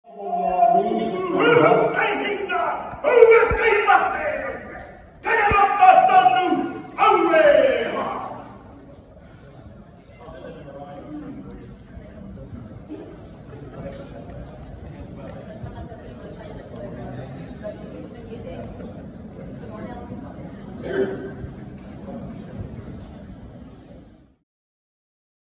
New Zealand MP performs brief haka in chamber before exit | AFP
Maori New Zealand lawmaker Rawiri Waititi performs haka after being ordered to exit the parliament chamber in Wellington. The Maori party co-leader was complaining about racism from the opposition's rhetoric regarding plans to set up a new Maori Health Authority to address health issues among the Indigenous people. Waititi was asked by the speaker to take his seat after his comments but instead he stepped out of his seat and performed haka, a ceremonial Maori dance to show resilience at challenging times.